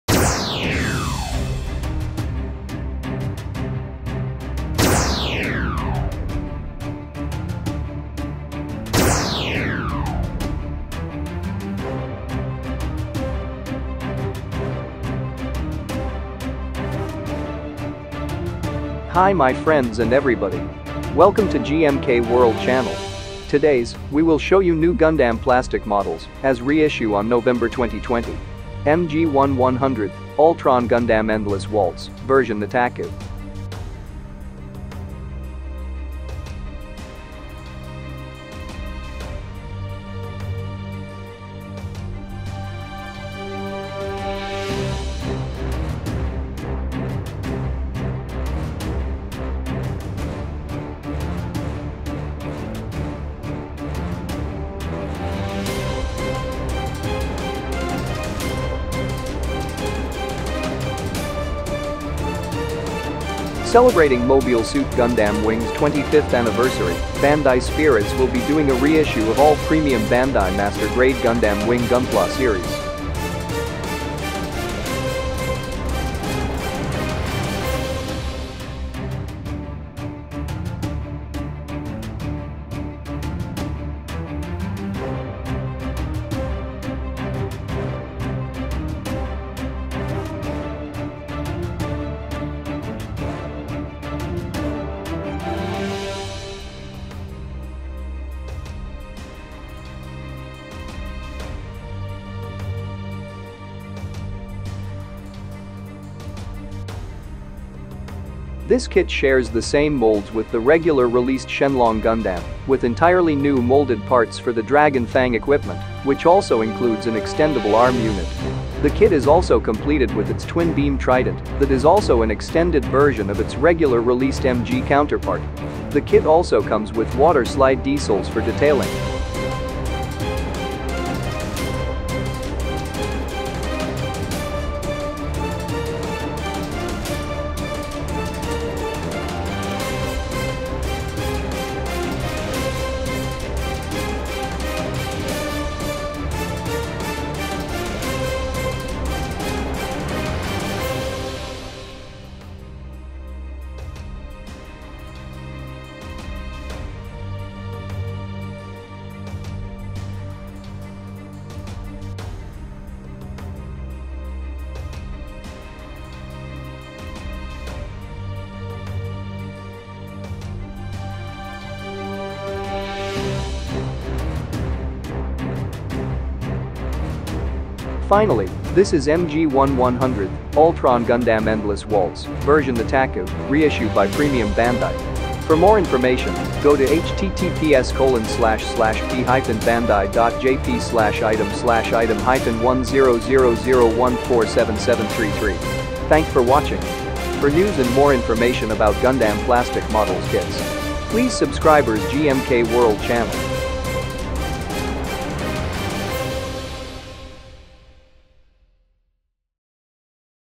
MG 1/100 Altron Gundam EW Ver. (Nataku) - Reissue(アルトロンガンダム ＥＷ)
Celebrating Mobile Suit Gundam Wing's 25th anniversary, Bandai Spirits will be doing a reissue of all Premium Bandai Master Grade Gundam Wing GunPla series.

This kit shares the same molds with the regular released Shenlong Gundam with entirely new molded parts for the dragon fang equipment which also includes an extendable arm unit. The kit is also completed with its Twin Beam Trident that is also an extended version of its regular released MG counterpart.

The kit also comes with water slide decals for detailing.